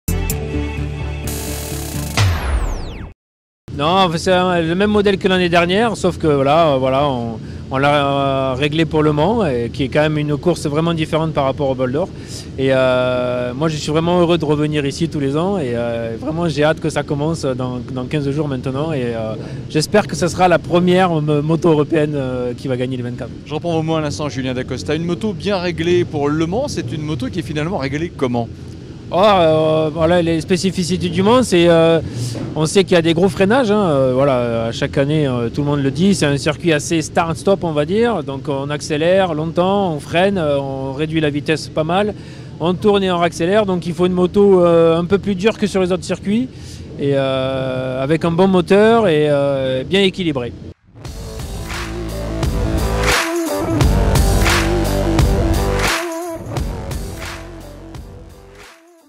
Pré-mans24 Heures Motos 2018 - Julien Da Costa – Tecmas BMW
Julien Da Costa a rejoint les rangs de l'équipe Tecmas-BMW en 2017. Le triple vainqueur des 24 Heures Motos n'a pas d'autre objectif que de permettre à la BMW S1000RR #9 de remporter la prochaine édition de la classique mancelle qui se déroulera les 21 et 22 avril 2018.

Official YouTube Channel of the "24 Heures Motos". The race 2018 will run from 21-22 April.